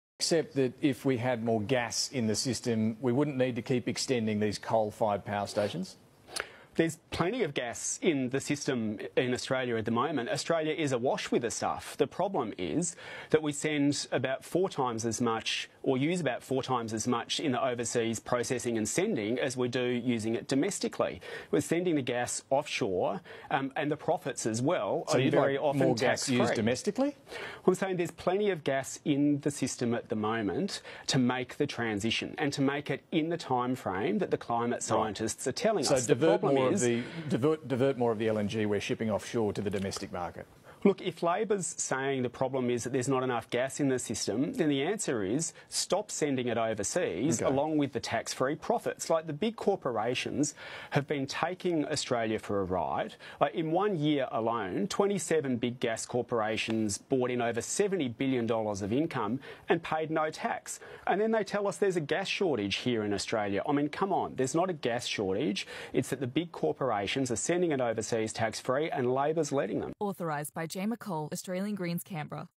There's no "gas shortage"
There’s no shortage of gas in Australia, just a shortage in Labor & Liberal politicians having the guts to take on the big corporations ripping us off.

Clip: ABC